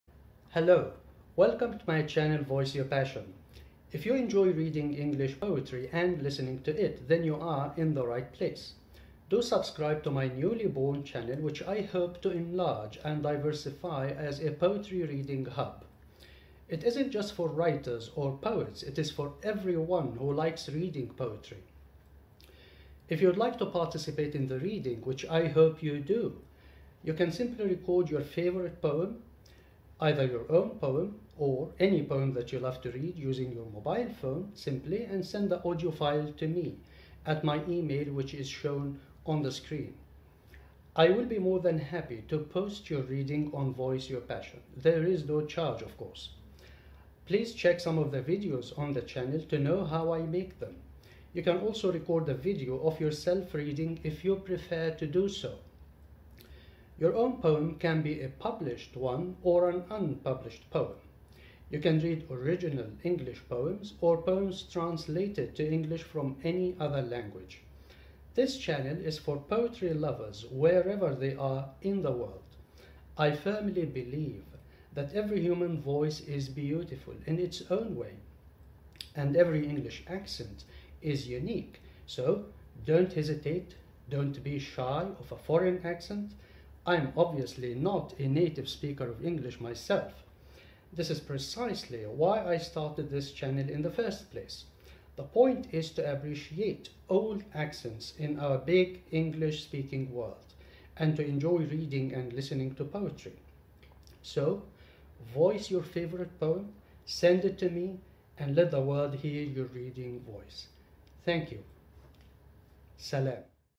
Introduction to Voice Your Passion!
If you enjoy reading English poetry or listening to it, then you’re in the right place. 'Voice Your Passion!' isn’t just for writers or poets; it is for everyone who likes reading poetry.

You can simply audio-record or video-record your favourite poem, either your own poem or any poem that you love to read, using your mobile phone, and send the file to me at my email address: . I will be more than happy to post your reading on 'Voice Your Passion!' (there is no charge of course). Please check some of the videos on the channel to know how they look.

Your own poem can be a published one or an unpublished poem. You can read original English poems or poems translated to English from any other language. This channel is for poetry lovers wherever they are in the world. I firmly believe that every human voice is beautiful in its own way, and every English accent is unique, so, don't hesitate! And Don’t be shy of a foreign accent! The point is to appreciate all accents in our big English-speaking world and to enjoy reading and listening to poetry.

So, voice your favourite poem, send it to me, and let the world hear your reading voice! Thank you!

Postscript - Some advice on audio recording :-
. Use any audio recording application on your mobile phone. There are many free ones on the Playstore or Apple store.
. Use your earphones or headphones while recording.
. Try to record in a quiet environment.
. Make sure to drink some water before starting to read.
. Keep the mic close to your lips to make the sound clear and display the beauty of your voice.
. Do not over-prepare yourself for the reading or take a deep breath or anything of the sort, just do it casually.
. Do not imagine standing in front of an audience. Imagine delivering an important message mouth-to-ear to people who are all ears and no eyes.
. Do not read too fast or too slow. Keep a pace you are comfortable with.